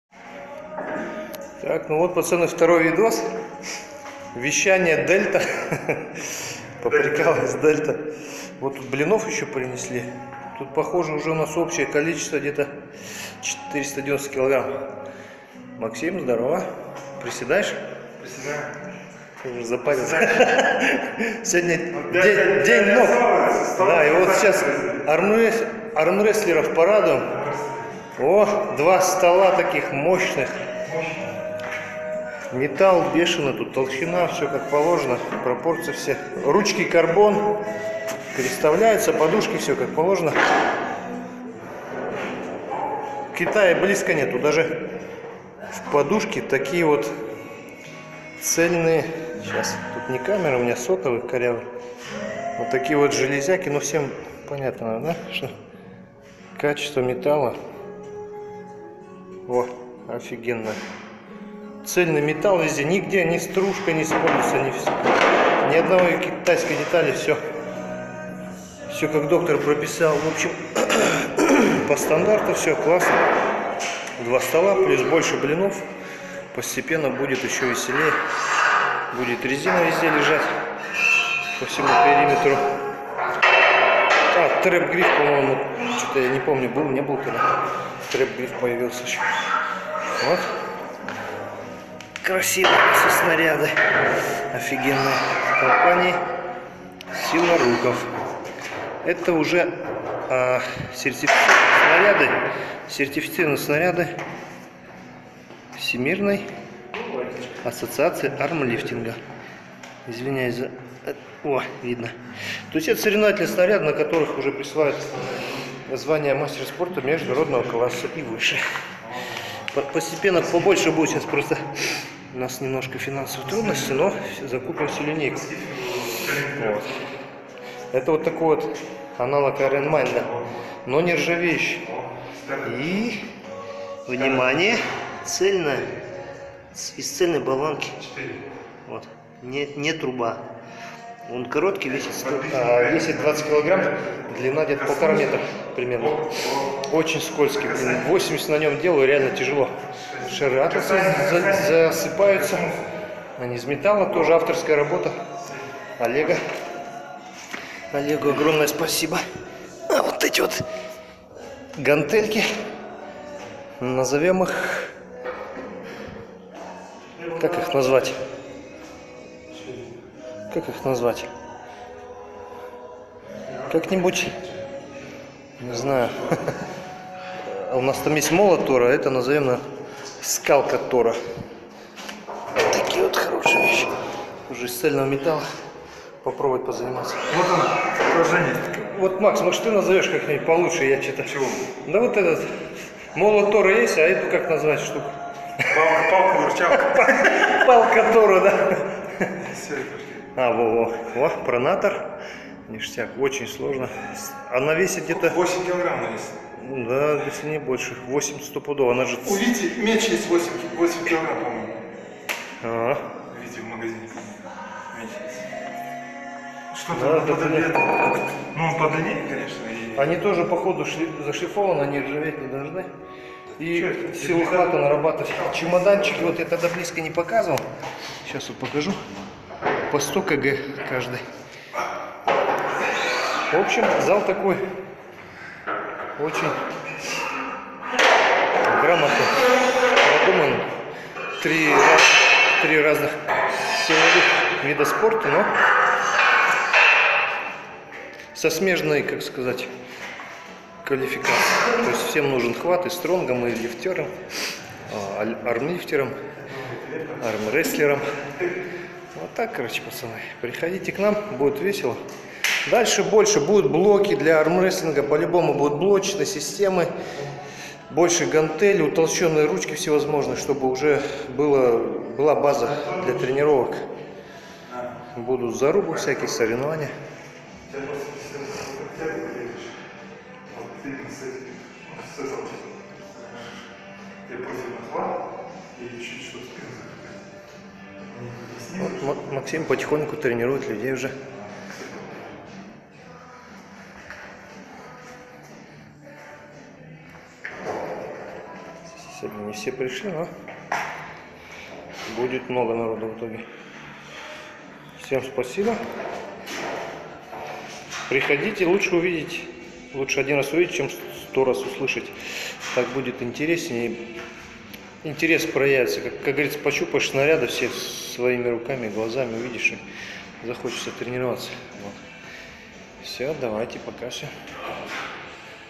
Второе видео из нашего силового зала
Второе видео нашего силового зала в г.Караганда .Железа в нашем силовом зале становится все больше и больше!
Приглашаем всех желающих в наш зал,для занятий армрестлингом, армлифтингом,и просто поддержать свою форму ! Все что вы увидите в нашем зале можно также заказать по выгодной цене.Все оборудование изготовлено на лазерном станке и из качественной стали.